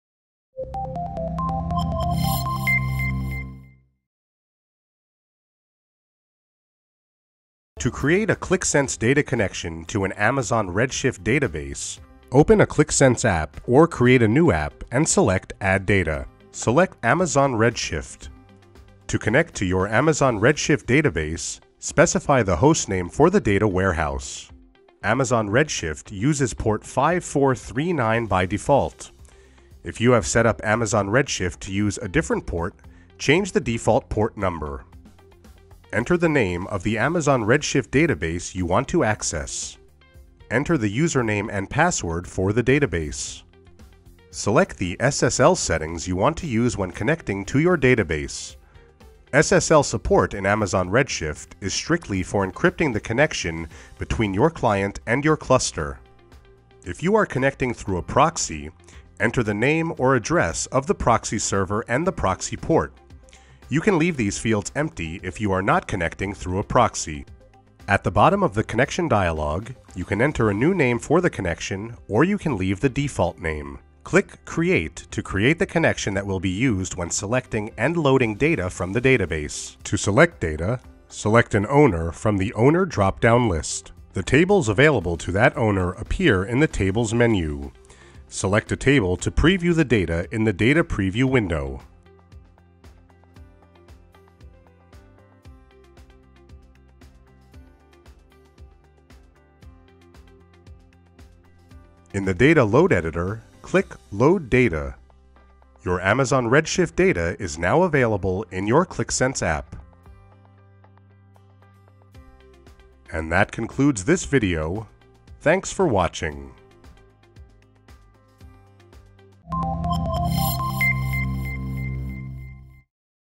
Connecting to Amazon Redshift - Qlik Cloud
This video shows you how to connect to Amazon Redshift database by creating a data connection with using Data load editor in Qlik Cloud